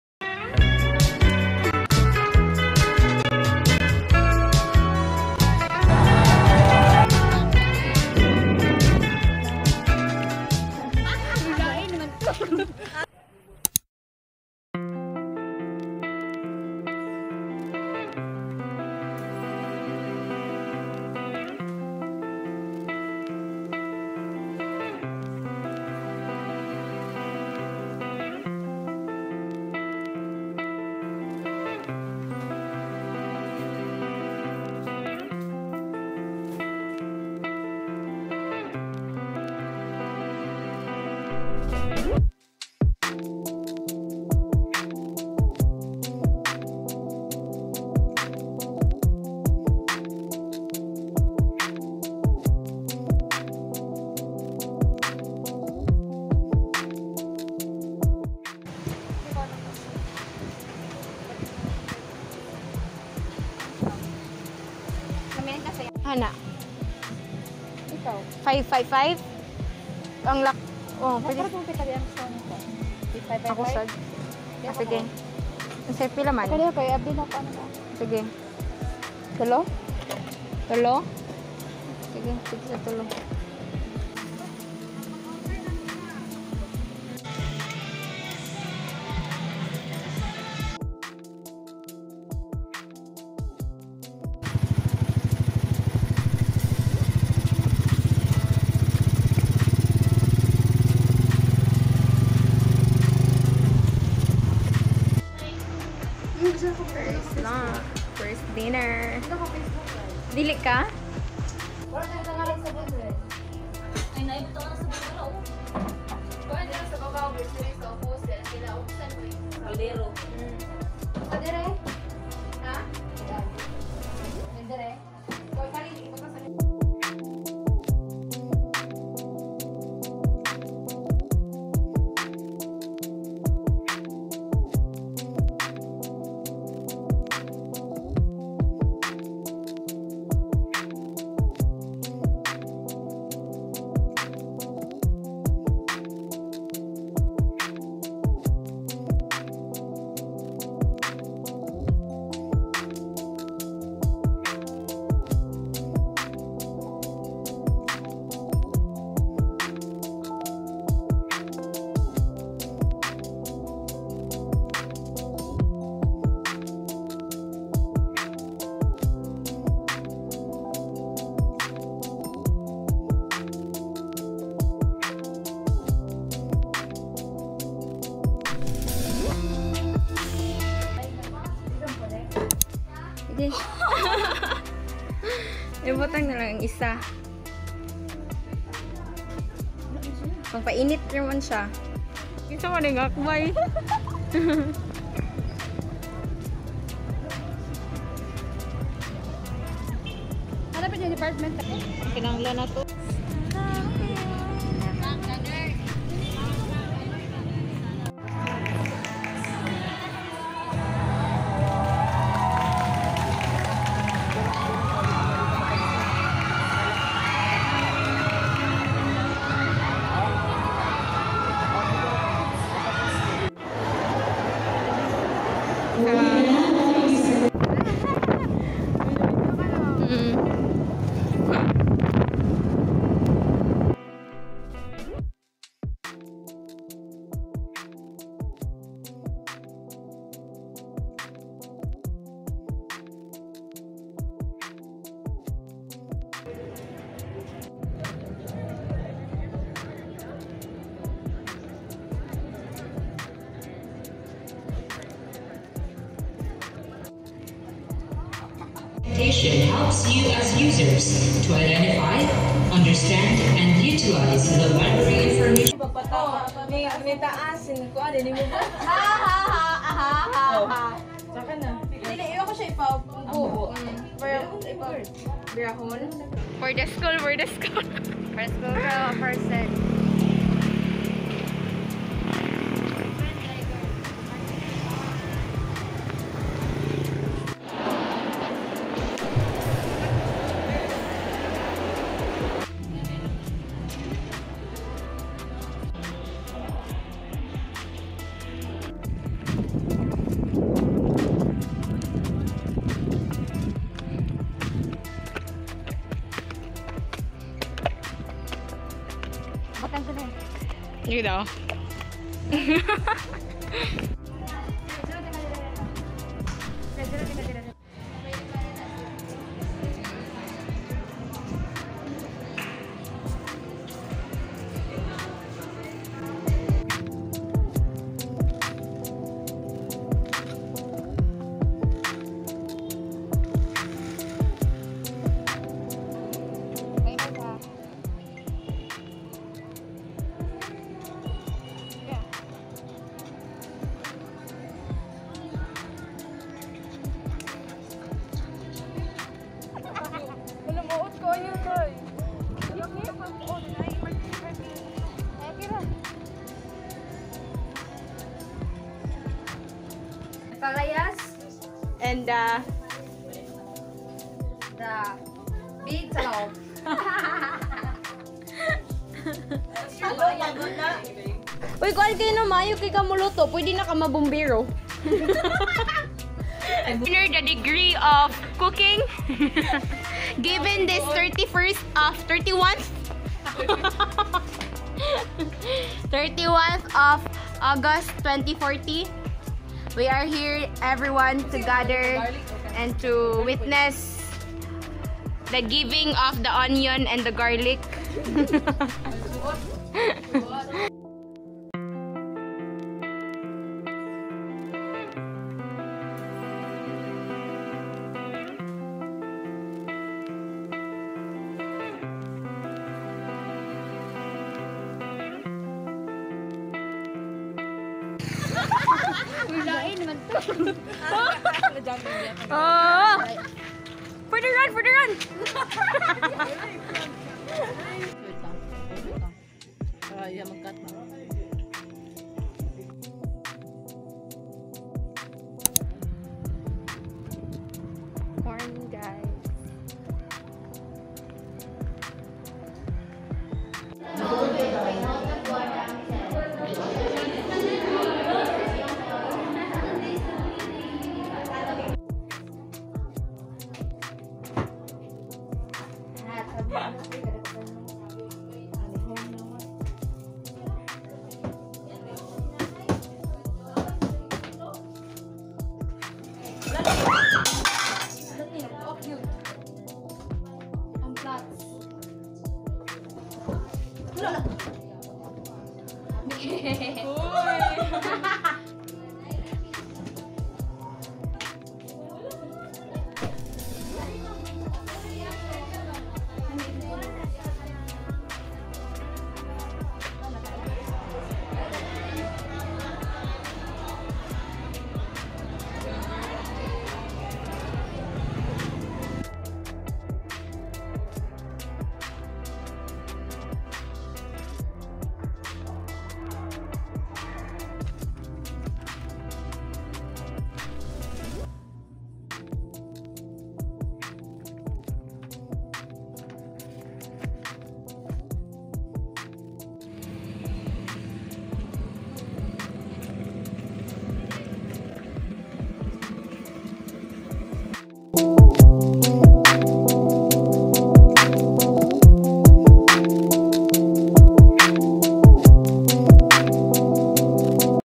college diaries (ep 3) ♡𐙚 moving in, boarding, back to school, productivity | Erin Rem
watch in 1080p | college vlog EP 3

editing apps used:
-vllo
-capcut